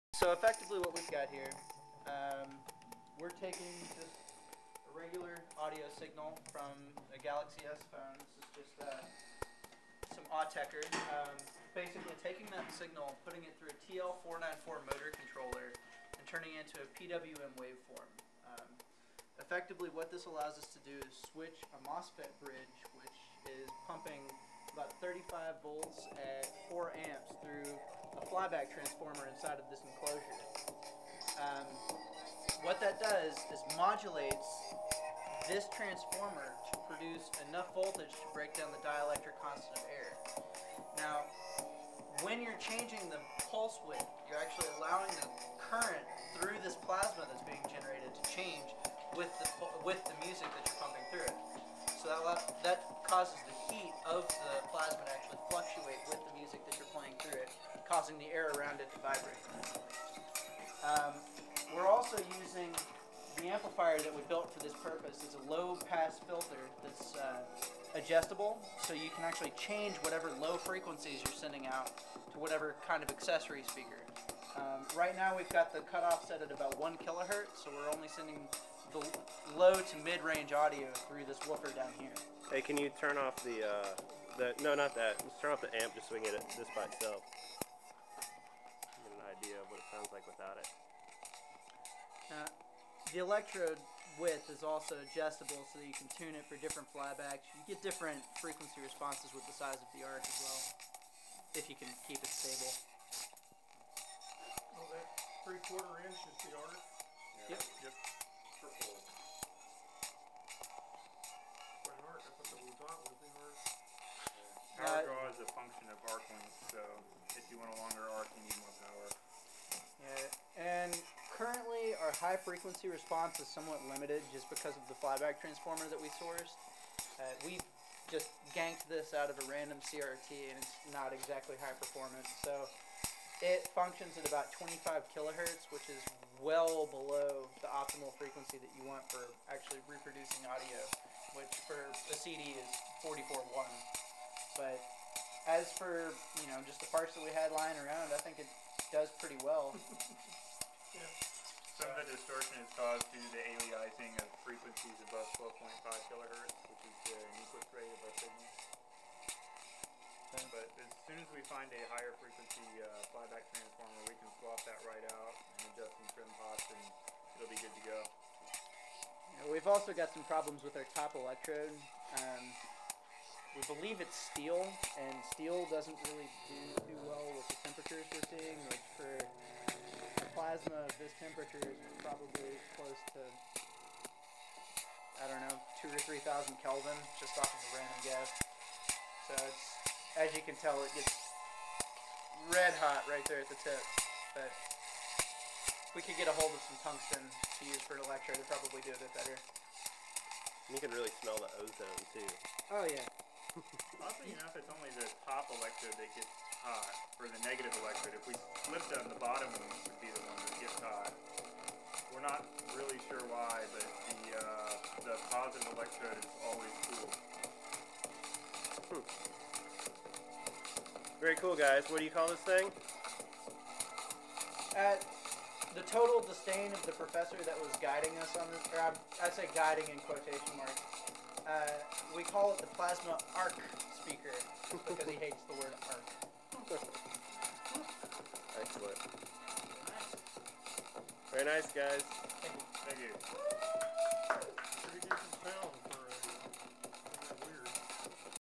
Plasma Arc Speaker
LVL1 Soundbuilders demonstration of a plasma arc speaker. Designed by Paul Faget and Seth Tucker.